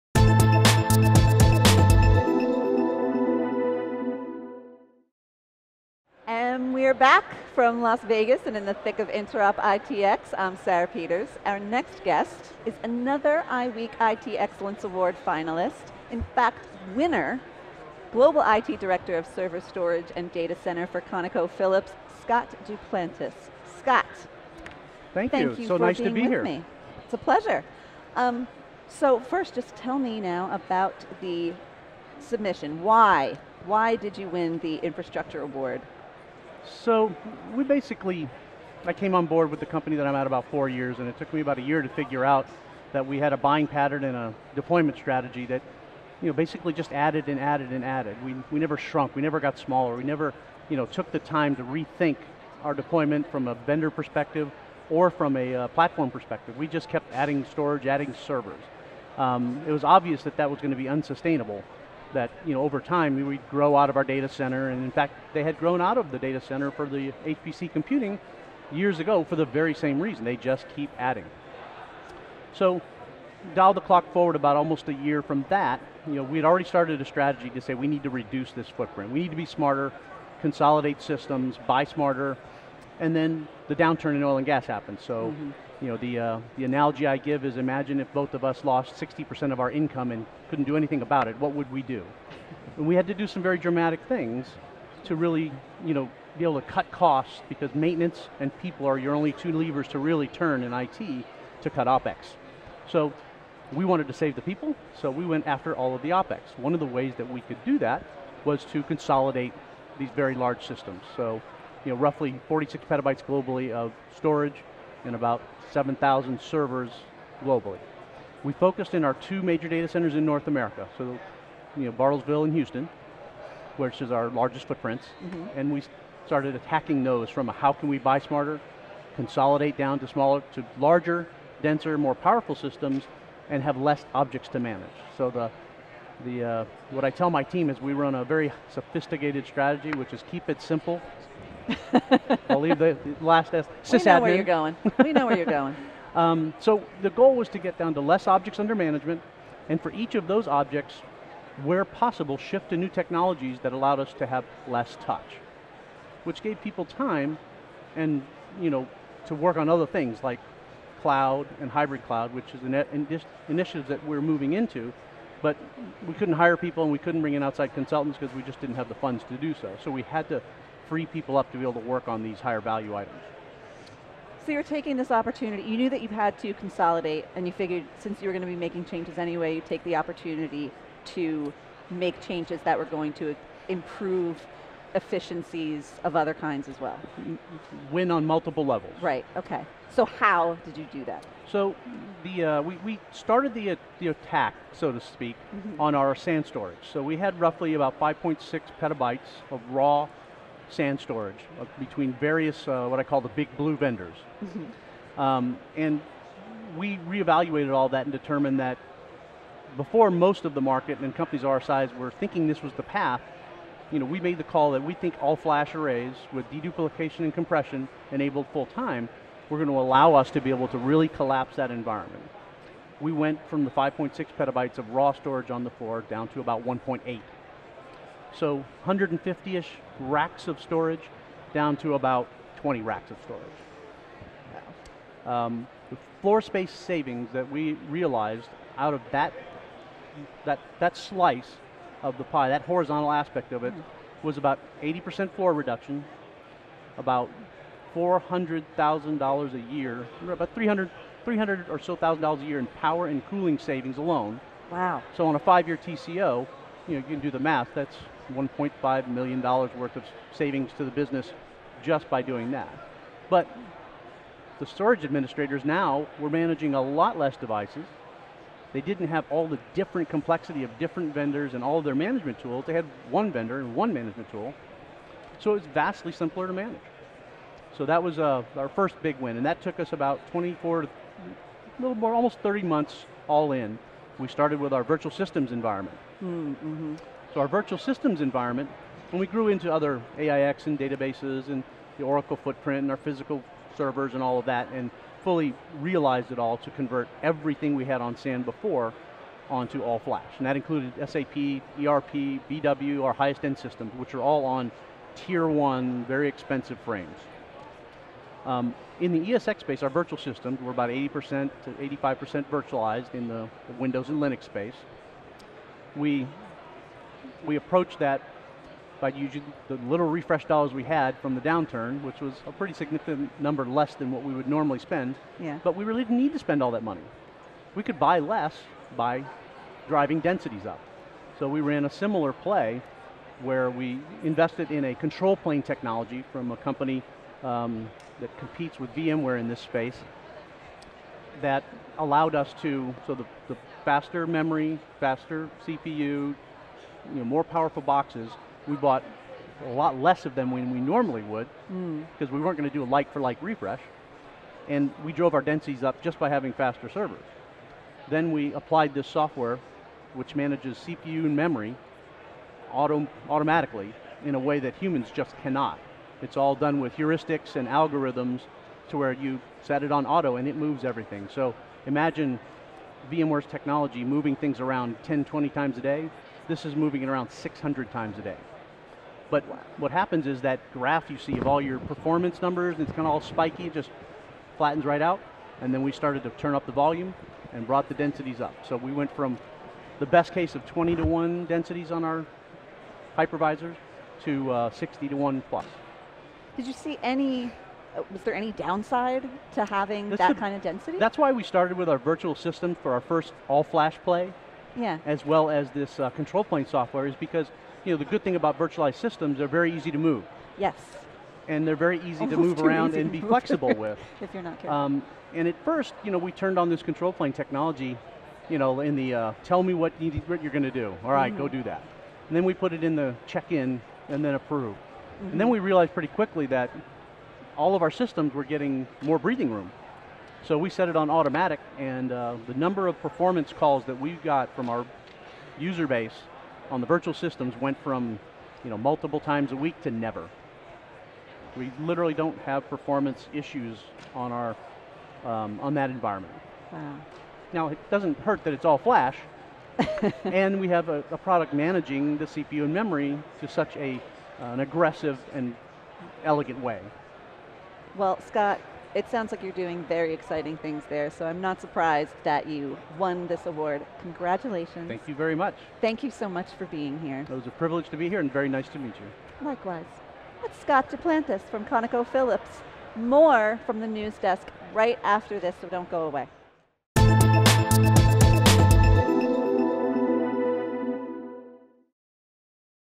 How ConocoPhillips Reduced its Data Center Footprint by 80%
Scott Duplantis, Global IT Director for server, storage, and data center operations at ConocoPhillips, talks about why his company's strategy to reduce its data center footprint won first place in the InformationWeek IT Excellence Awards infrastructure category.

Duplantis says that ConocoPhillips' IT department was suffering from an unsustainable buying pattern and deployment strategy that was too quick to add more storage and servers. The IT group knew it wanted to start buying smarter and consolidate to more powerful systems, while shifting to new technologies that would free up time for other projects like cloud and hybrid cloud. Due to an untimely economic downturn, they had to do all of this on a tight budget, and refused to cut jobs. Watch the video above to learn how ConocoPhillips was able to save $300K in power and cooling a year, decrease raw storage on the floor by 3.8 PB, and reduce its floor by 80% -- all in about 30 months.